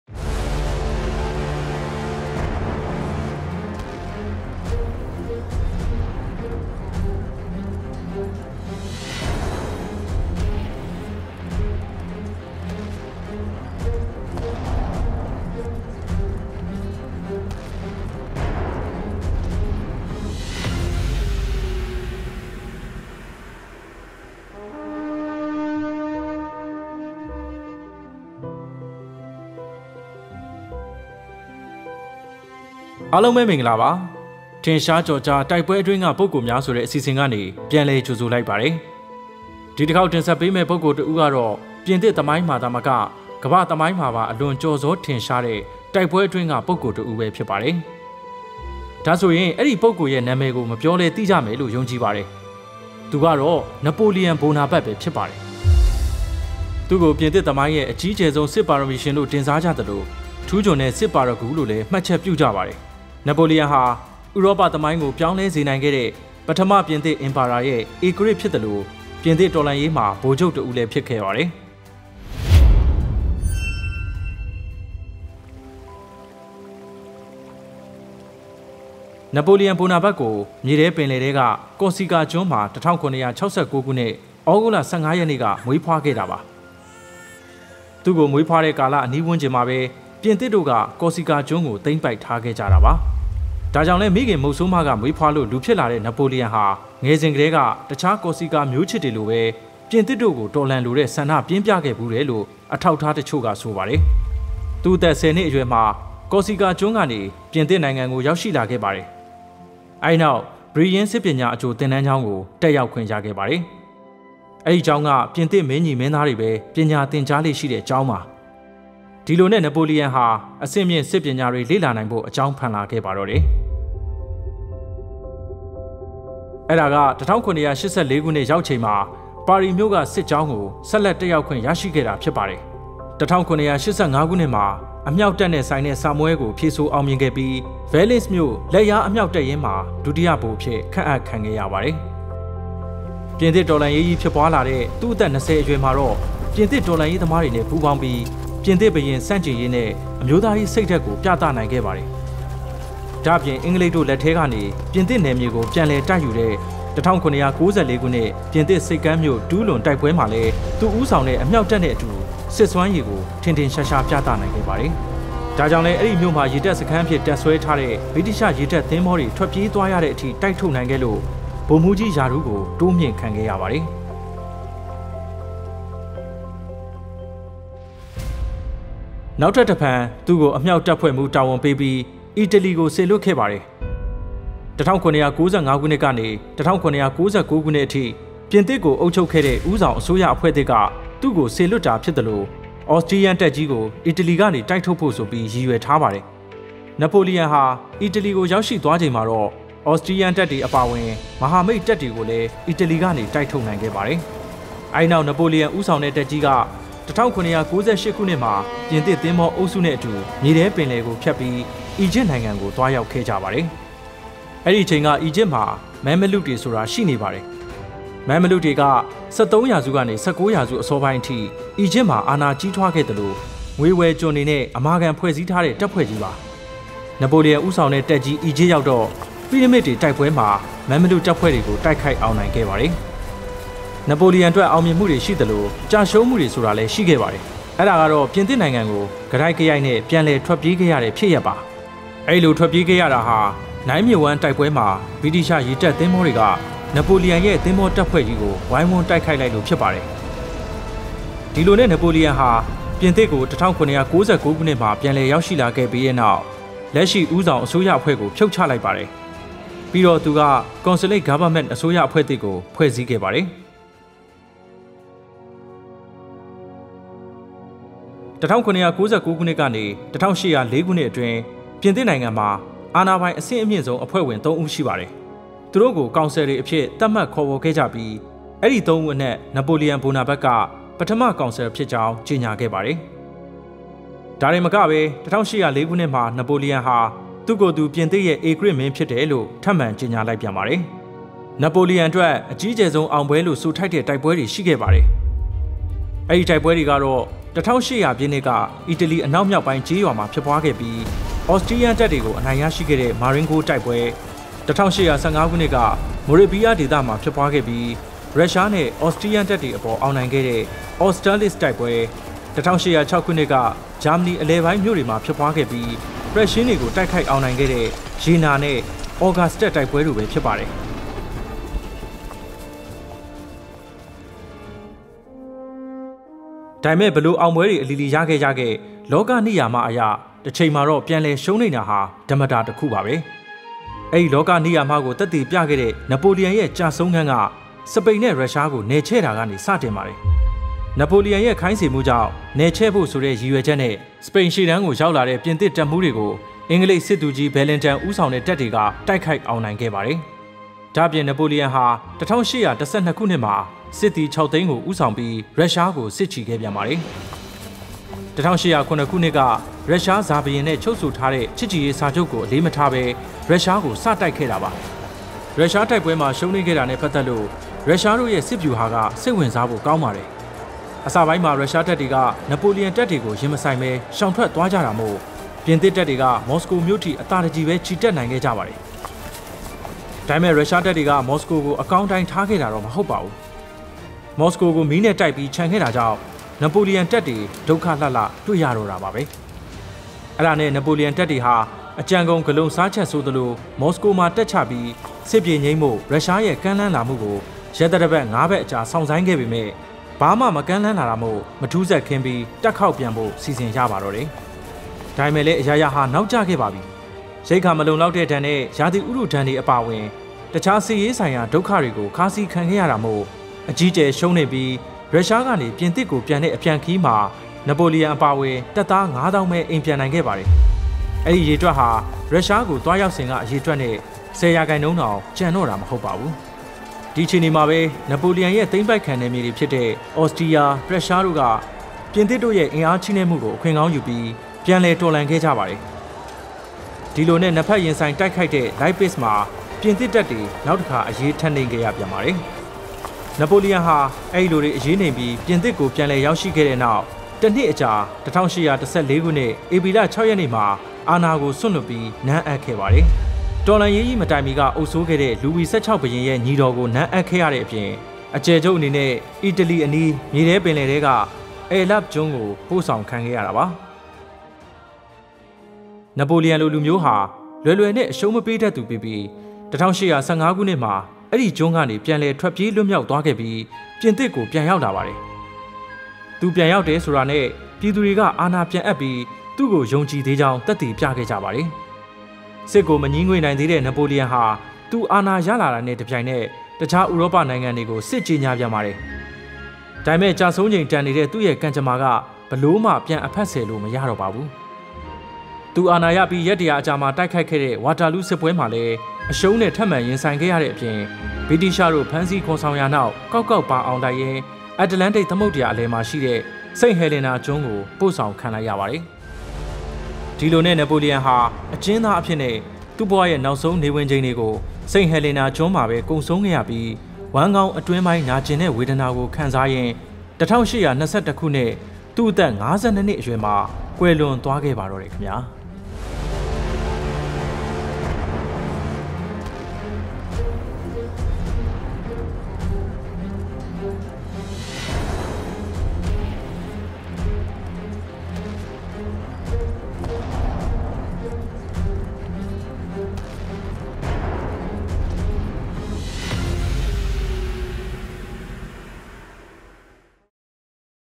နပိုလီယန်
စစ်ပွဲအတွင်းမှ ထင်ရှားပုဂ္ဂိုလ်တွေထဲမှာတော့ ပြင်သစ်သမိုင်းမှာသာမက ကမ္ဘာ့သမိုင်းမှာပါအလွန်ထင်ရှားခဲ့ပြီး စစ်ဘုရင်တစ်ဦးအဖြစ်သတ်မှတ်ခံခဲ့ရတဲ့ "နပိုလီယန်ဘိုနာပတ်" ကိုလည်းချန်ထားခဲ့လိုမရပါဘူး။🙋

နပိုလီယန်ဟာဆိုရင် ဥရောပသမိုင်းကိုပြောင်းလဲစေနိုင်ခဲ့တဲ့ ပထမပြင်သစ်အင်ပါယာရဲ့ ဧကရာဇ်ဖြစ်သလို ပြင်သစ်တော်လှန်ရေးမှာလည်း ဗိုလ်ချုပ်တစ်ဦးအဖြစ်တာဝန်ထမ်းဆောင်ခဲ့သူဖြစ်ပါတယ်။ ဒီတစ်ခေါက် "ထင်ရှားကျော်ကြားတိုက်ပွဲတွင်းကပုဂ္ဂိုလ်များ" အစီအစဥ်မှာတော့  ပြင်သစ်သမိုင်းရဲ့အကြီးကျယ်ဆုံးစစ်ပါရမီရှင်လိုတင်စားကြတဲ့ နပိုလီယန်ရဲ့ ငယ်ဘဝနဲ့ စစ်ဗျူဟာတွေ အပြင် နောက်ကွယ်ကသမိုင်းကြောင်းတွေကိုပါ တင်ဆက်ပေးလိုက်ပါတယ်။ 🤩